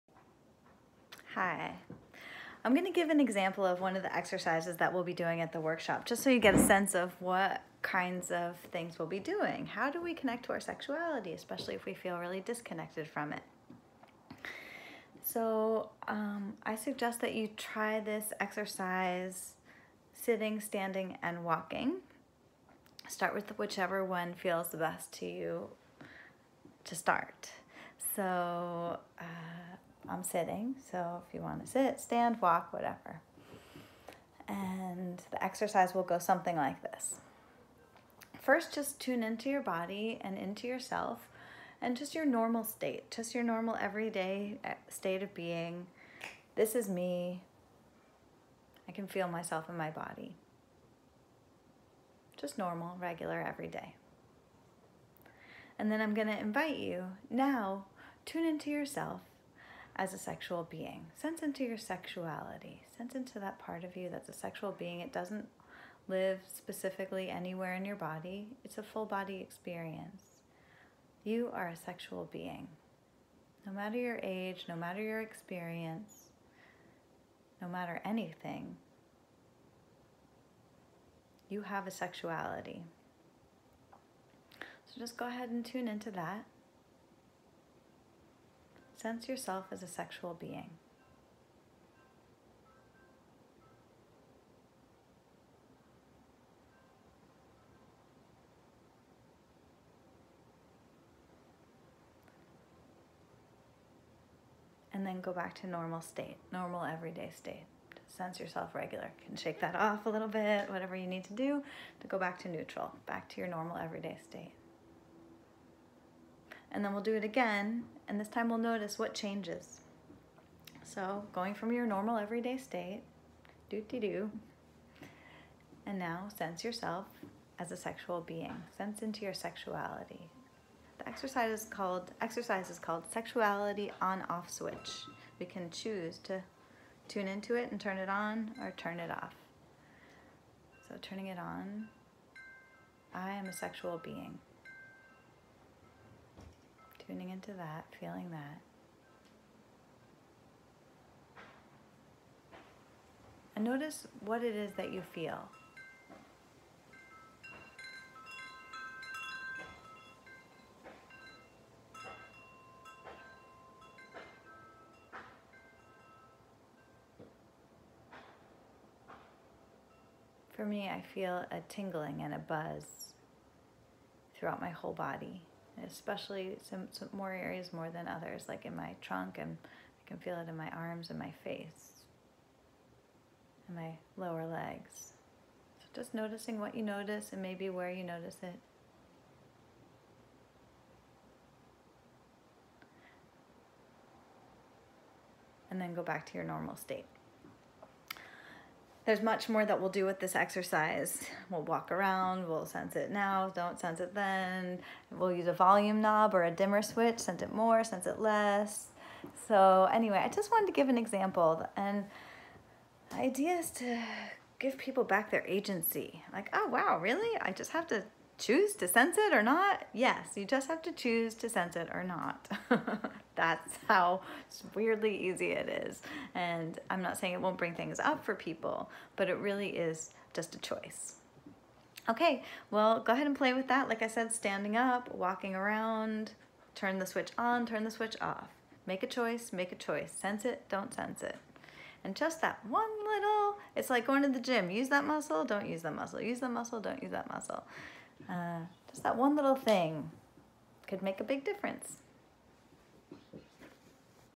Sexuality On/Off switch
This is an example of one of the exercises we will be doing as a part of my workshop, "Connecting to your Sexual Self" on Sunday July 28th in Eugene, Oregon.

The workshop is intended to help people reconnect or connect more deeply to their own sexuality. There will be no partnering exercises, only solo exploration which will include meditation, movement and journaling.